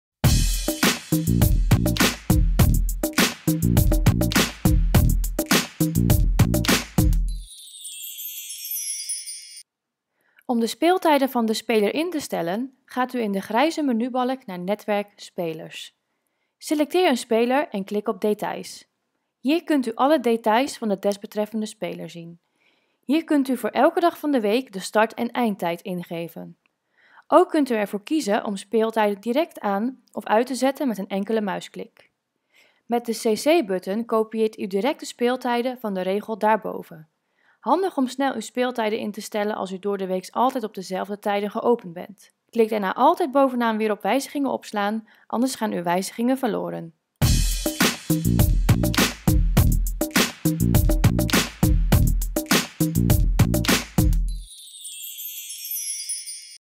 Speeltijden speler instellen
Er bestaan systemen waarbij je vrolijk elke ochtend en elke avond met de afstandsbediening langs moet lopen. Uiteraard niet met het TINS systeem van Merit Media. De speler zorgt namelijk automatisch voor de inschakeling én uitschakeling van uw monitoren op de door u gewenste tijdstippen!